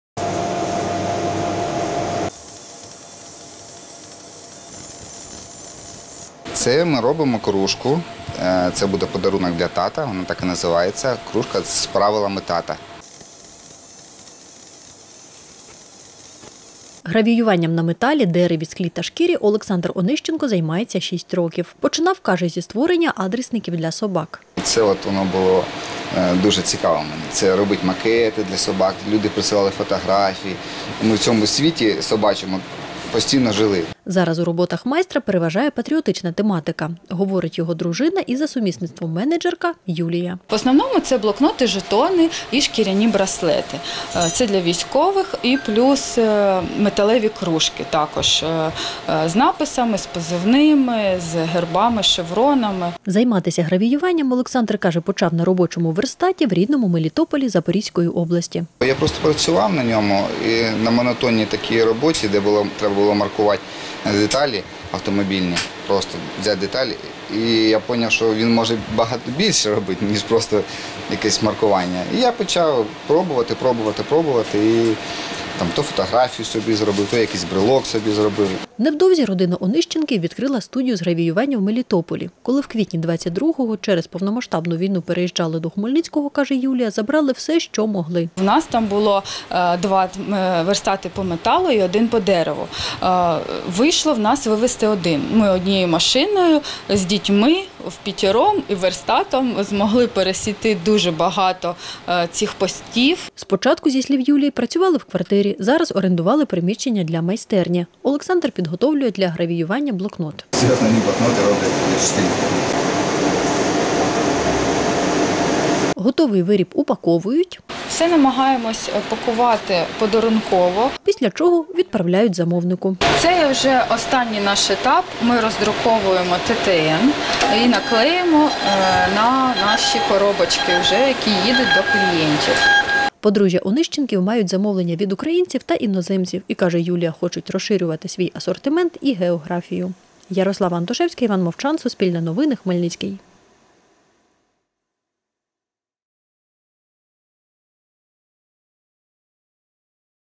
Подружжя з Мелітополя відкрило у Хмельницькому майстерню з гравіювання
Майстерню з гравіювання відкрило в Хмельницькому подружжя Онищенків з Мелітополя Запорізької області. З рідного міста Юлії та Олександру вдалося вивезти один верстат. На новому місці спочатку працювали вдома, нині ж облаштували майстерню в орендованому приміщенні.